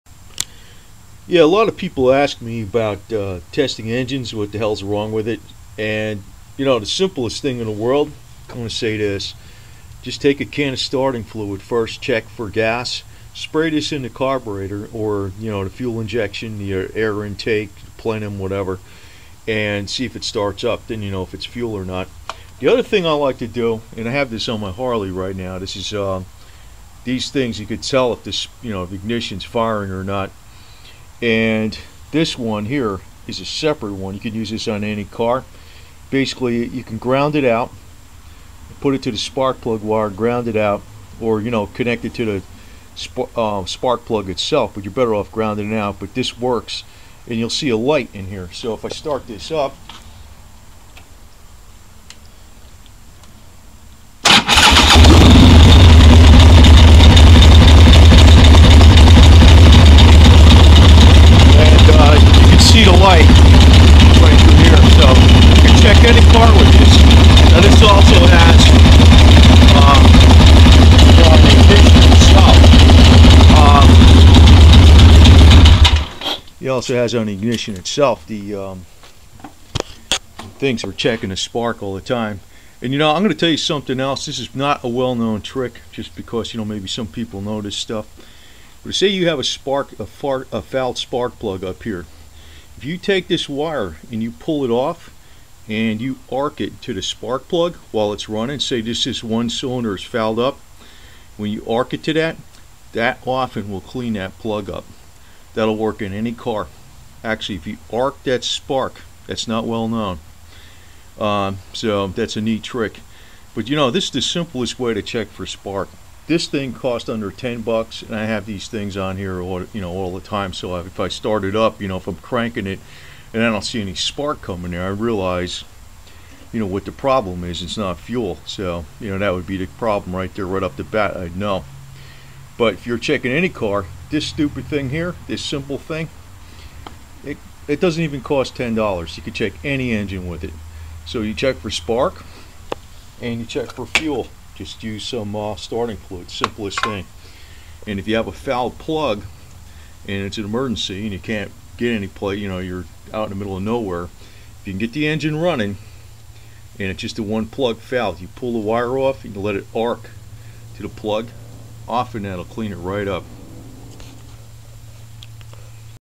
Quick How to - Engine Troubleshooting- SPARK & FUEL
A very quick how to on checking for fuel and spark.  All engines basically work the same.  This is the simple fast way to start isolating the problem.
  If you can not start your car or your car just died or you car is stranded on the road, use these simple automotive diagnostic tips to determine where the engine problem is.